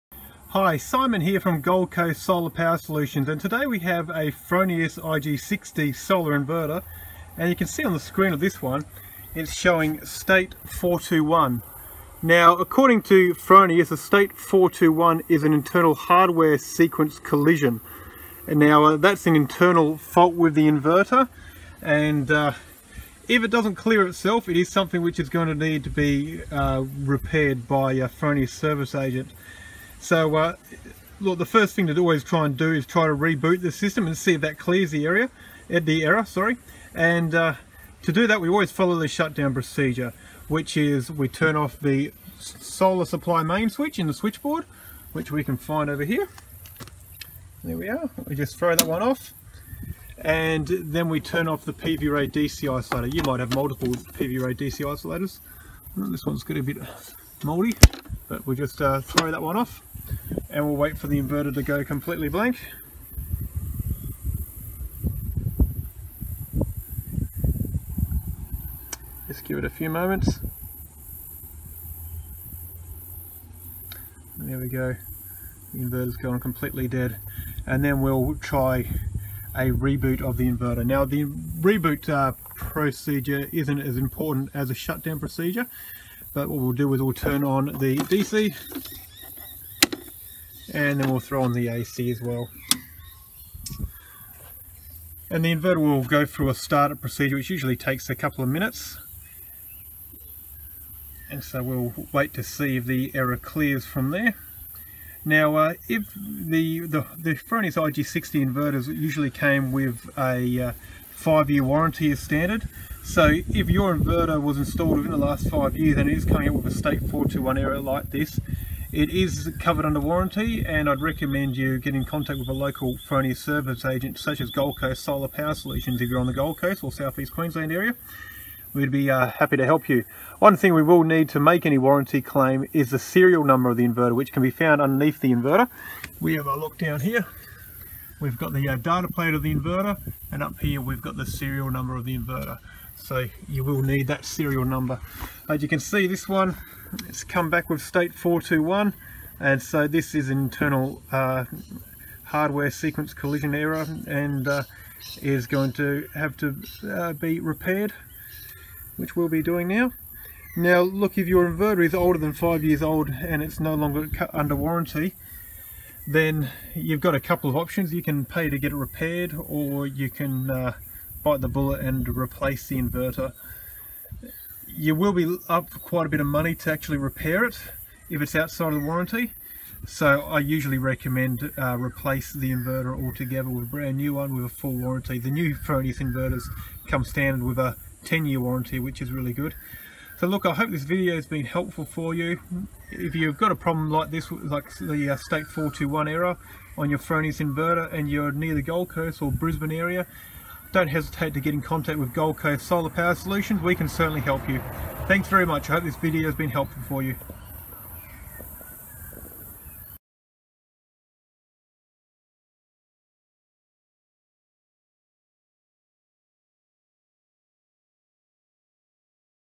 Fronius STATE 421 Error Message
Is your Fronius inverter displaying a STATE 421 error message on the screen? Find out what it means and what you can do about it here with Gold Coast Solar Power Solutions.